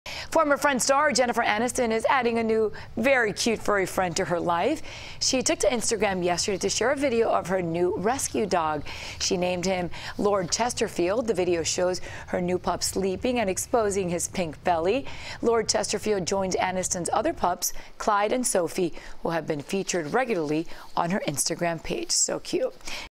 Trending: Jennifer Aniston's New Dog
Former "Friends" star Jennifer Aniston has adopted a rescue dog.